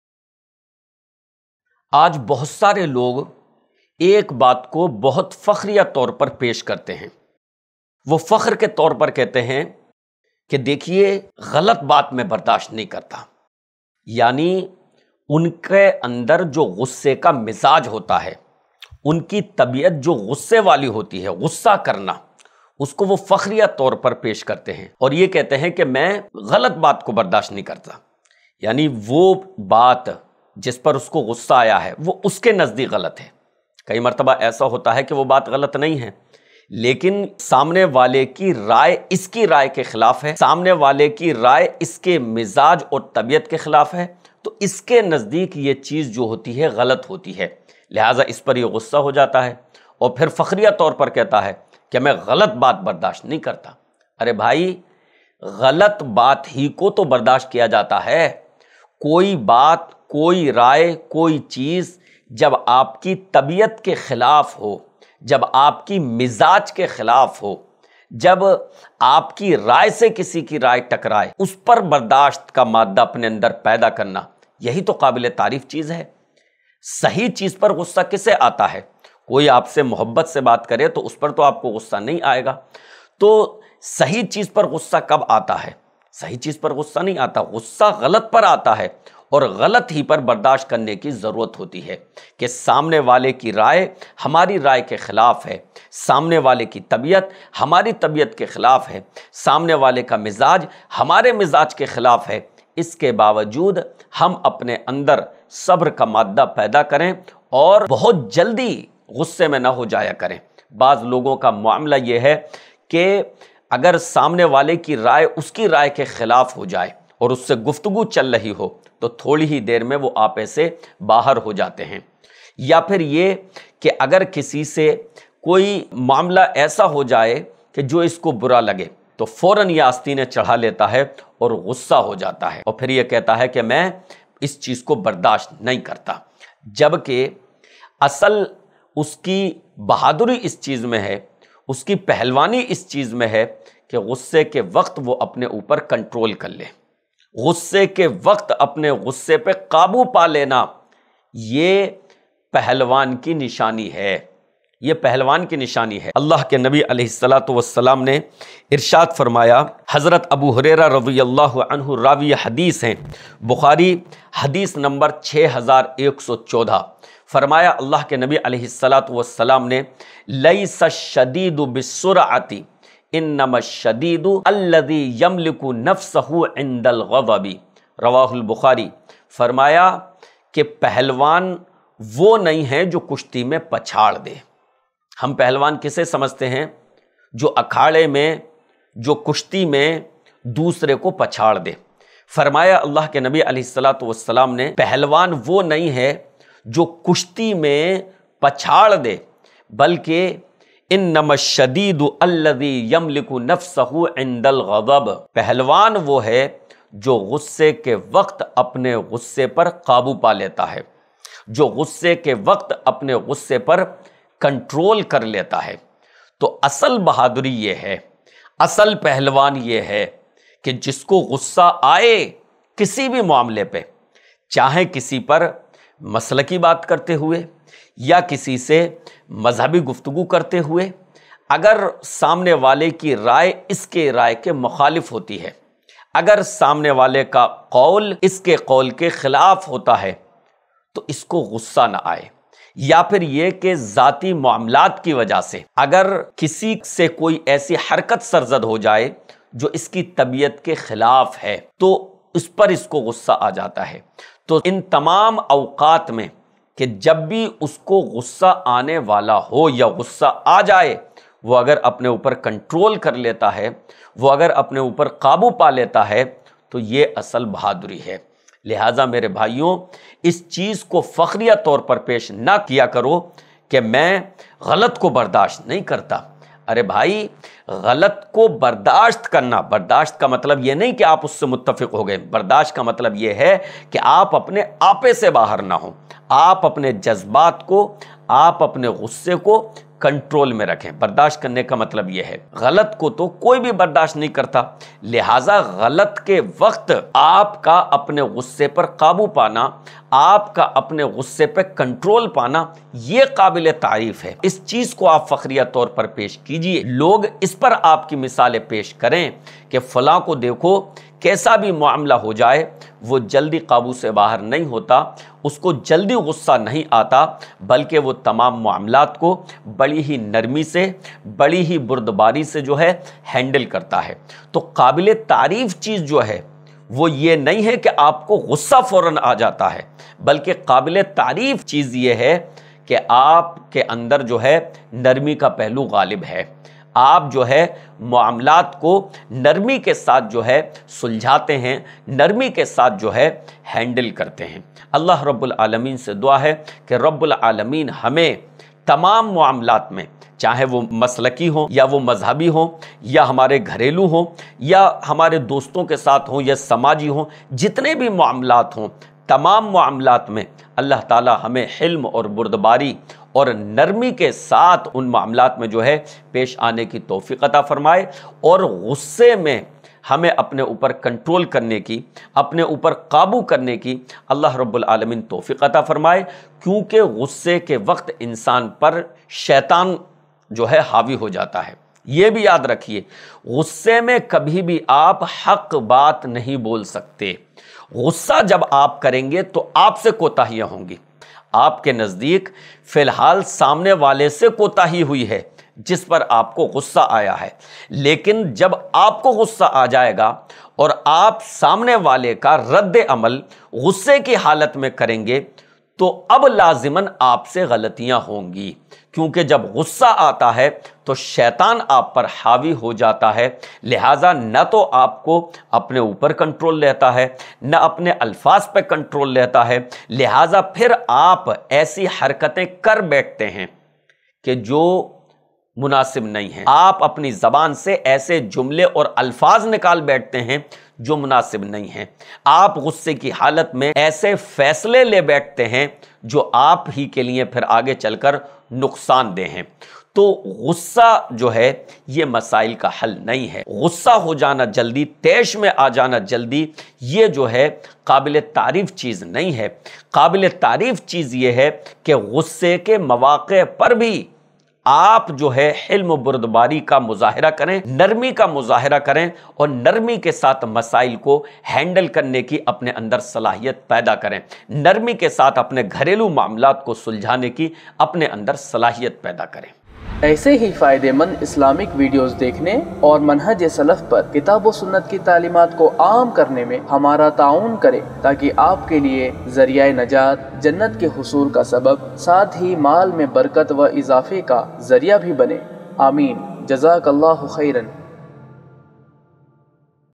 Gussa Mat Karo Kyonki Gussa Karne Se.. By Shaikh Kamaluddin Sanabili | IIC Mumbai
Bank Account Details:
ILM FOUNDATION
ICICI Bank
Savings account
IFSC CODE-ICIC0001028

Gussa Mat Karo Kyonki Gussa Karnaa... By Shaikh Kamaluddin Sanabili | IIC Mumbai